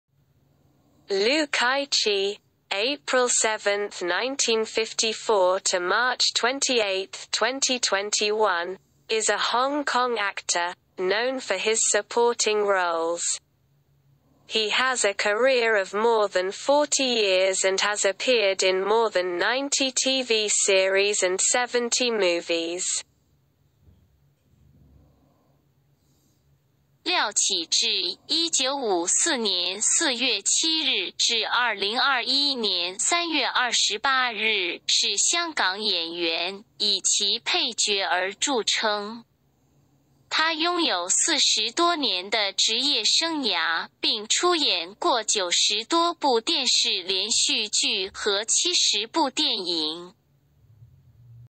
廖启智Liu Kai-chi (April 7, 1954 to March 28, 2021) is a Hong Kong actor, known for his supporting
Liu Kai-chi (April 7, 1954 to March 28, 2021) is a Hong Kong actor, known for his supporting roles. He has a career of more than 40 years and has appeared in more than 90 TV series and 70 movies.

廖启智（1954年4月7日至2021年3月28日）是香港演员，以其配角而著称。 他拥有40多年的职业生涯，并出演过90多部电视连续剧和70部电影。